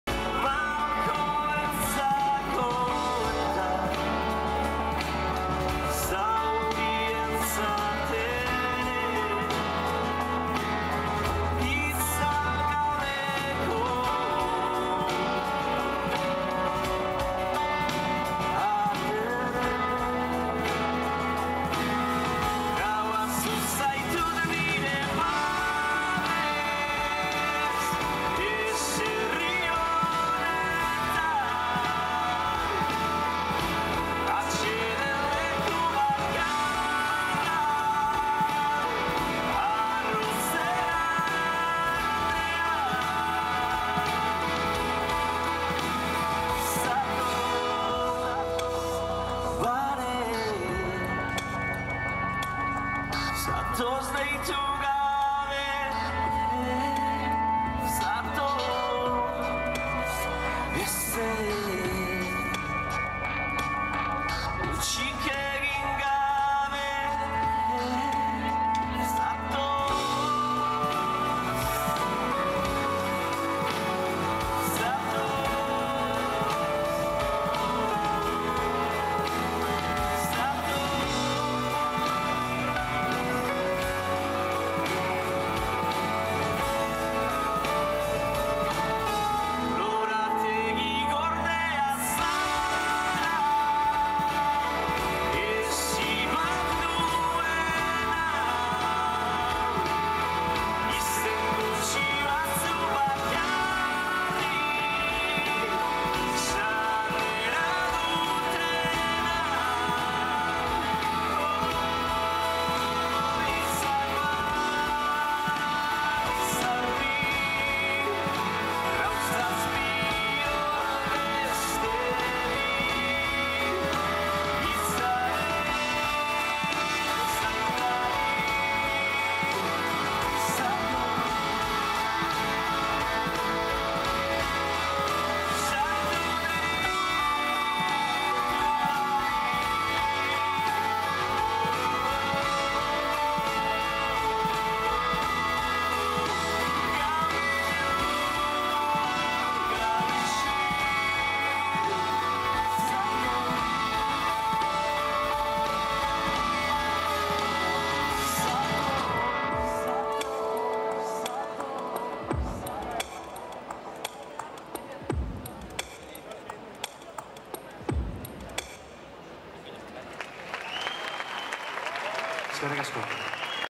Concierto de Eñaut Elorrieta en Bilbao
Eñaut Elorrieta hizo las delicias de los seguidores que se dieron cita ayer por la noche en la Plaza Nueva de Bilbao.